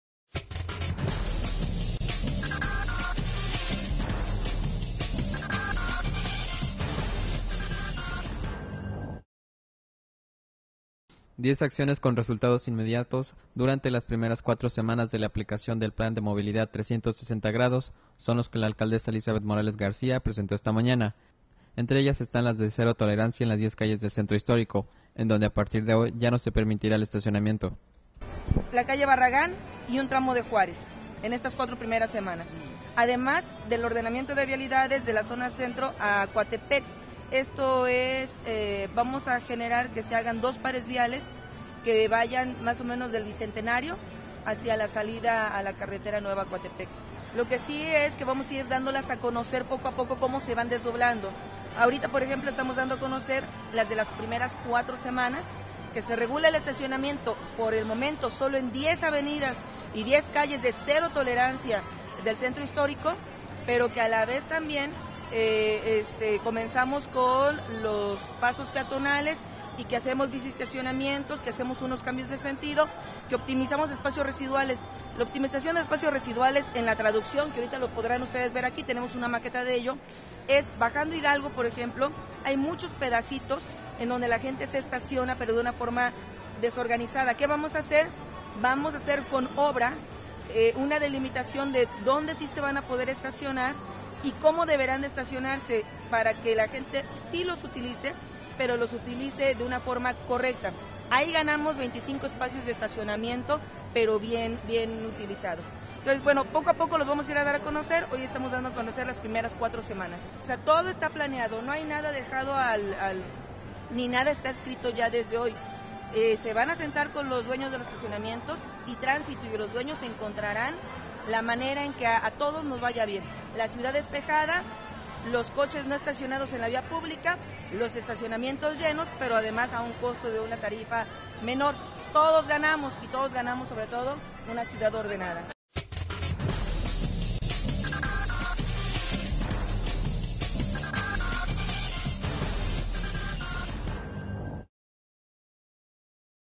Cero tolerancia en calles del centro de Xalapa por Plan de Movilidad 360º: Elízabeth Morales
Diez acciones con resultados inmediatos durante las primeras cuatro semanas de la aplicación del Plan de Movilidad 360º, son las que la alcaldesa Elízabeth Morales García presentó esta mañana; entre ellas están la cero tolerancia en 10 calles del centro histórico, en donde, a partir de hoy, ya no se permitirá el estacionamiento.